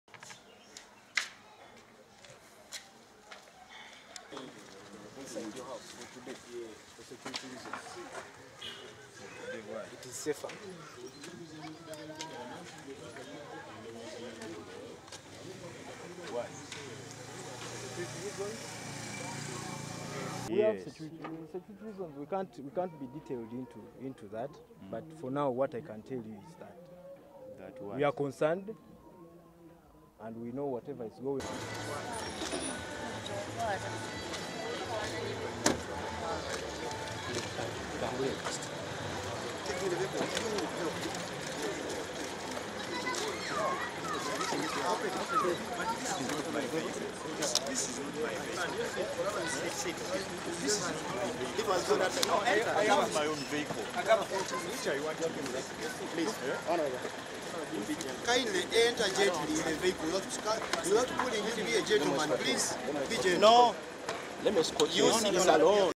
Police foil opposition rally, arrest MP Ssewanyana
Police in Kampala have arrested the Makindye West MP, Allan Ssewanyana for reportedly attempting to defy their order that sought to stop him from attending a banned prayer service at the city suburb of Katwe. The legislator was arrested at his home early this morning and taken to an unknown destination